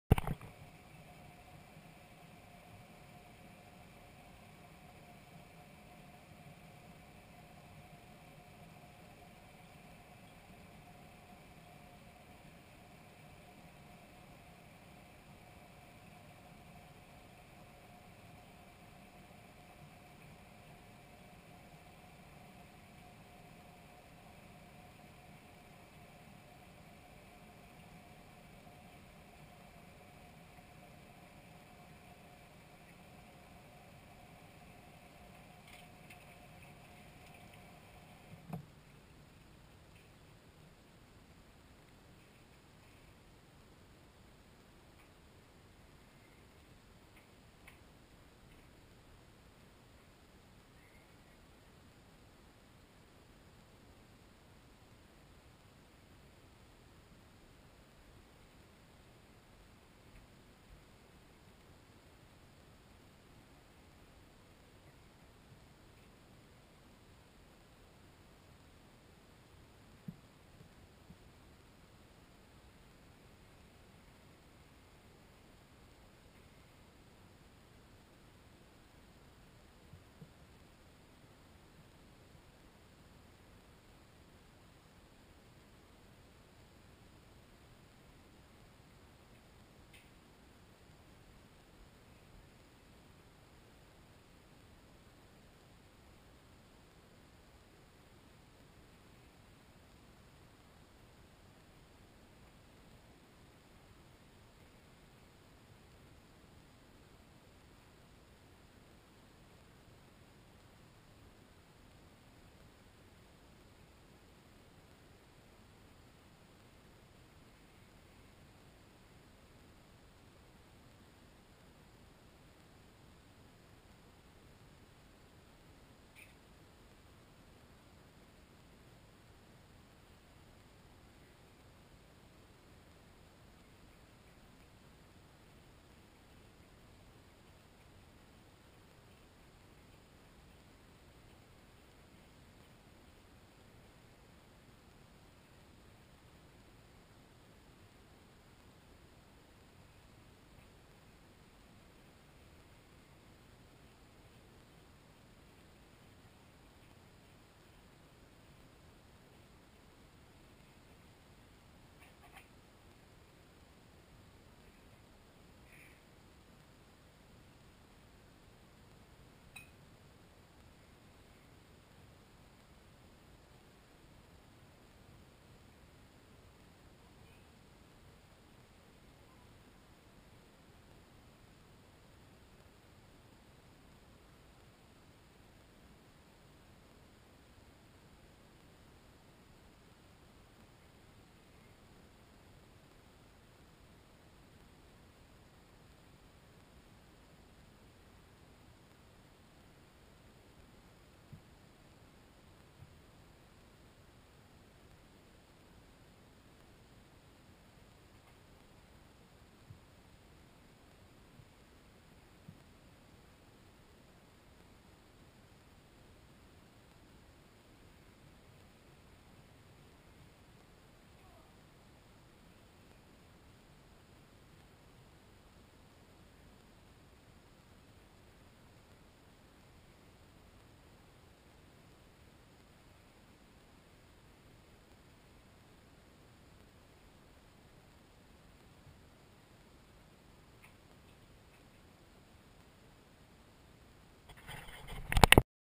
steve the cat eats fruit
Steve eats watermelon, blackberries, and strawberries